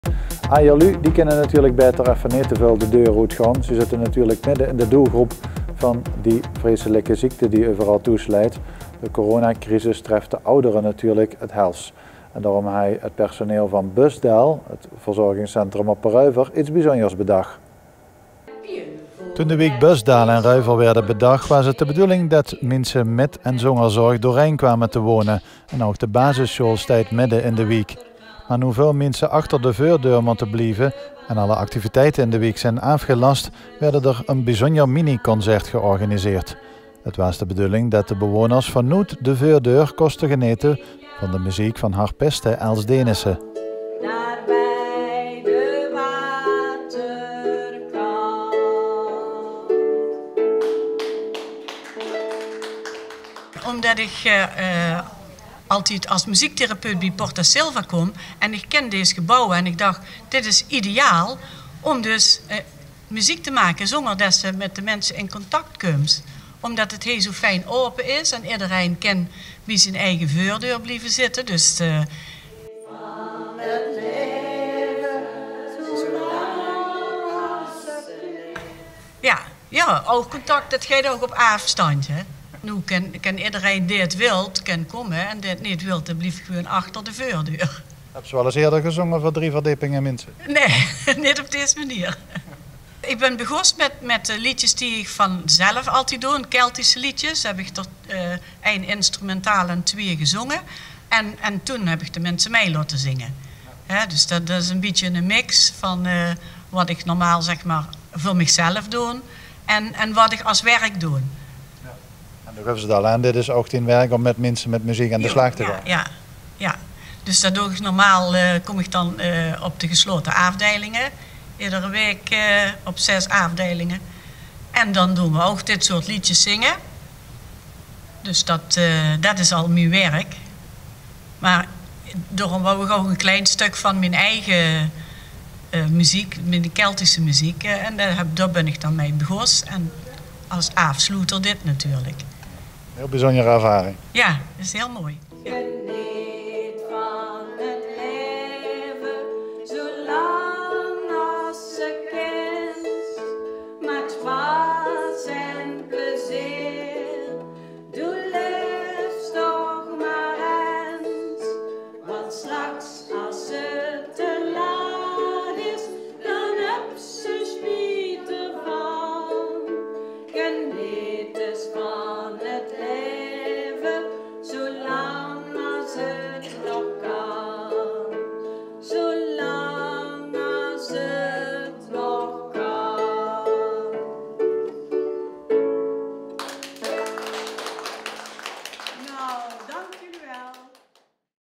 TVEllef: Voordeurconcert voor bewoners Bösdael Reuver
Vanaf de eigen voordeur genieten van livemuziek, bewoners van Bösdael die vanwege coronacrisis niet samen mogen komen werden getrakteerd op een bijzonder concert.